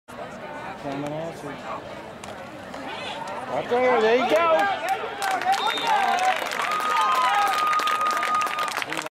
2013 AHS 9th v Bartlett #2
A stop by the the Tiger's defense.  Bartlett had a couple tough running backs.